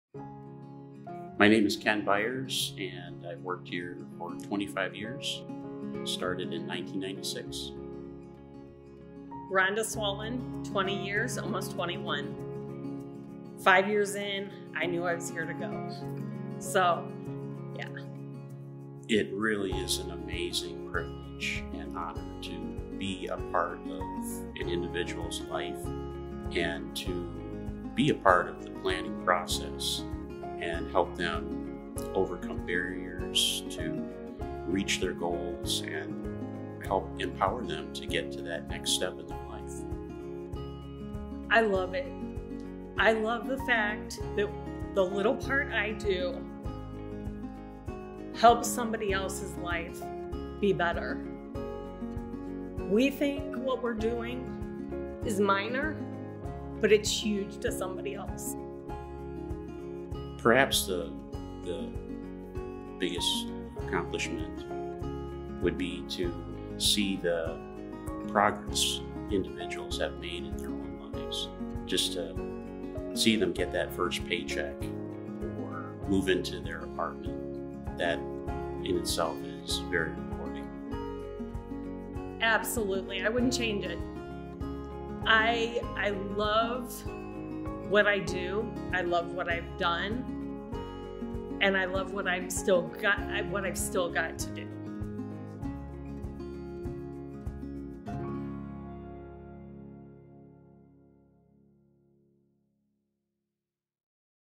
Thanking Staff 2021: The Heart of Summit DD Week 4
Throughout the month of November, we spotlight Summit DD staff with more than 20 years of service! Join us for our week four celebration as we honor Ken Byers and Rhonda Swallen.